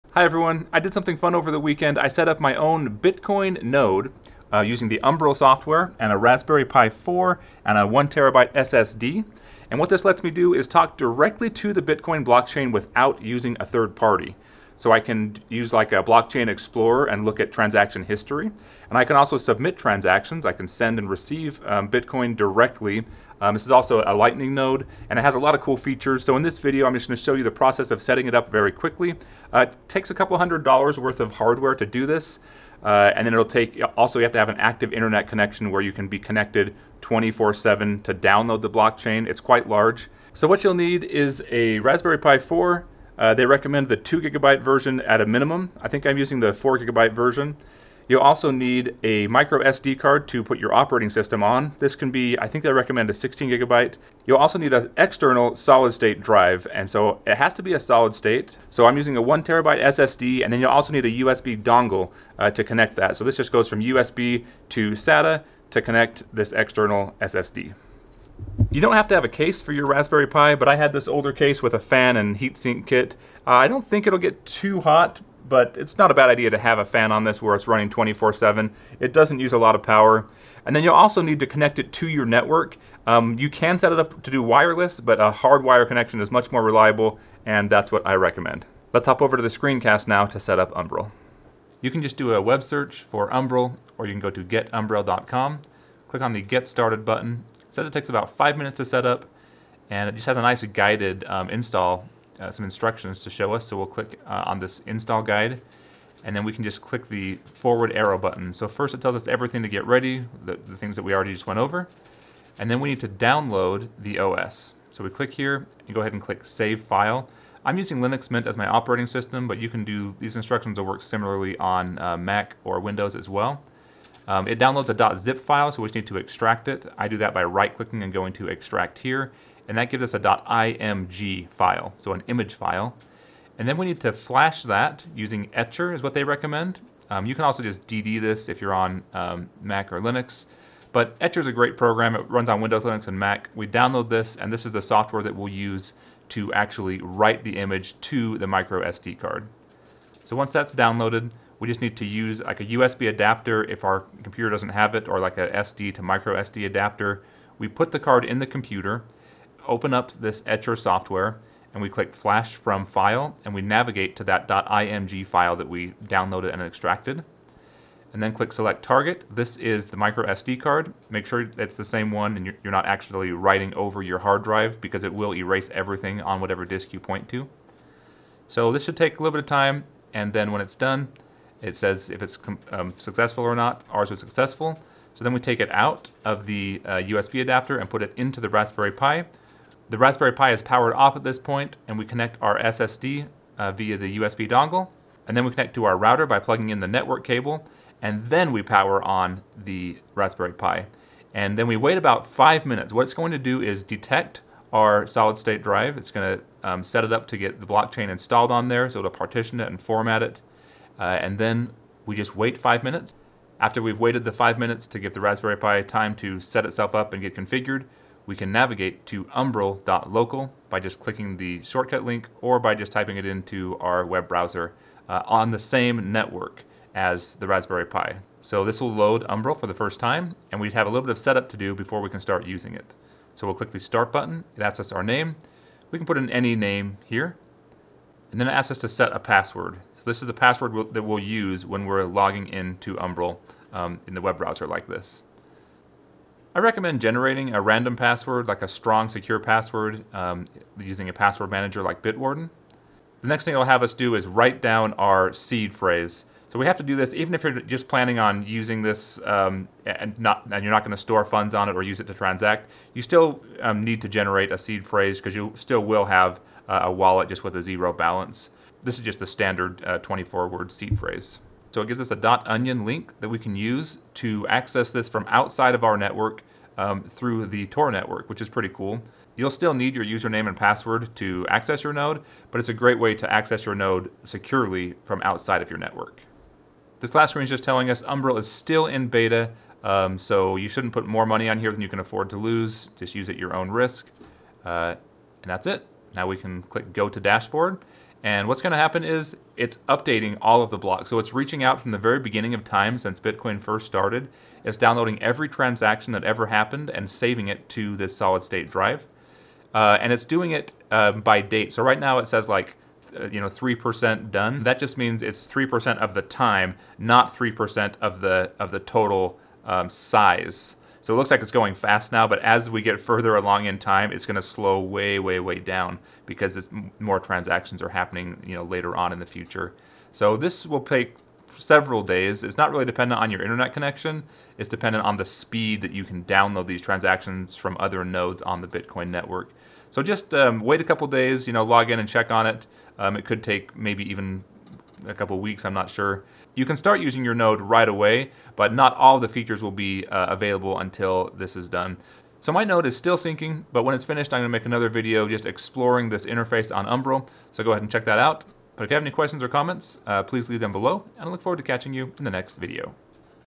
Umbrel Setup | Raspberry Pi Bitcoin & Lightning Node
Umbrel lets you run a Bitcoin and Lighting node on the Raspberry Pi 4. It is easy to setup, and comes with a well designed user GUI / graphic interface that you can access from any web browser within your local network and/or outside your network via TOR.

Raspberry Pi 4 - 4gb Kit

SATA to USB Cable -

1TB SSD

USB to microSDXC adapter

For those interested, this is the exact acrylic case and fan I am using